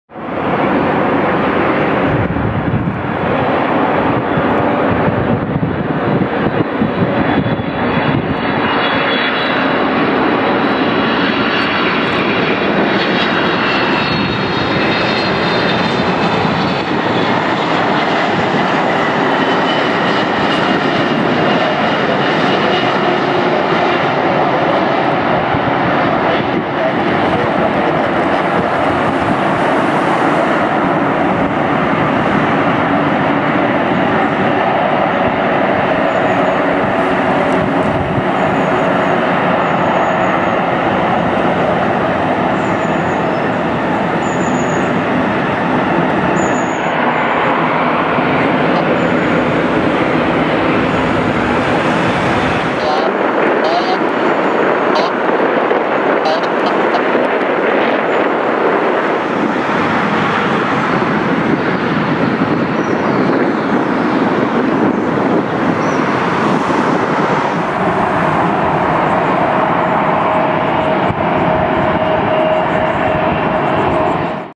Boeing 777-300 landing shortly before the rain! Hamburg Airport A6-EGR
Boeing 777-300ER Emirates Airlines touch down at Hamburg Airport RWY 33. A6-EGR